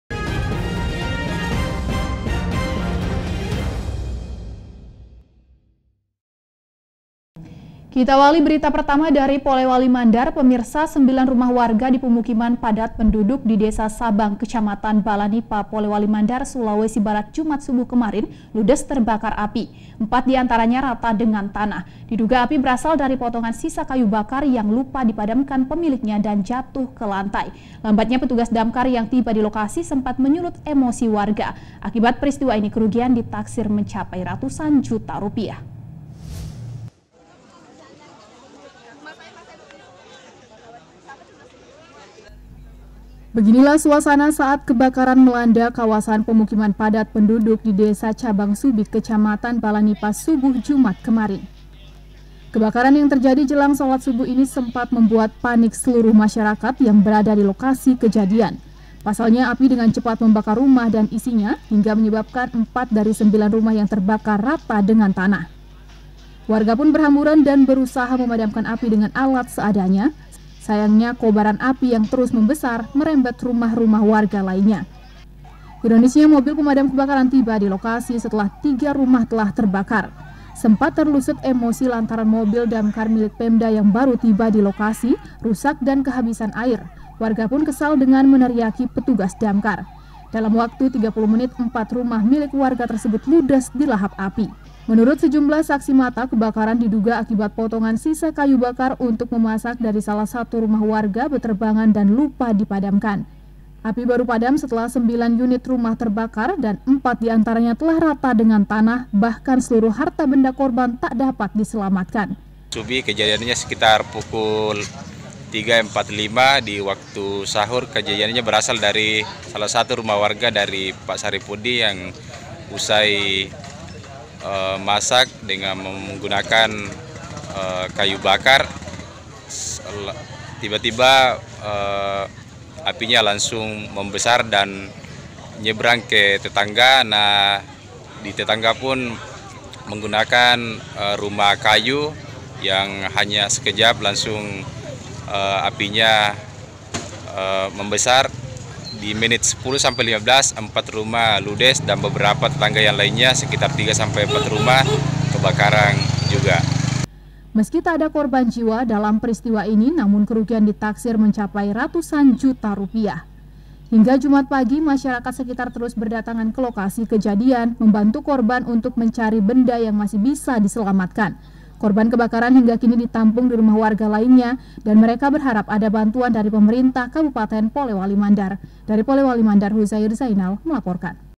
Kebakaran Di Perkampungan Padat | iNews Sulbar | 27-05-2019
Sembilan rumah warga dipemukiman padat penduduk,di dessa sabang, kecamatan balanipa/polewali mandar,sulawesi barat jumat subuh kemarin, ludes terbakar api, empat diantaranya rata dengan tanah. Diduga api berasal dari potongan sisa kayu bakar  yang lupa dipadamkan pemiliknya dan jatuh ke lantai. Lambatnya petugas damkar yang tiba dilokasi sempat menyulut emosi warga. Akibat peristiwa ini kerugian ditafsir mencapai ratusan juta rupiah.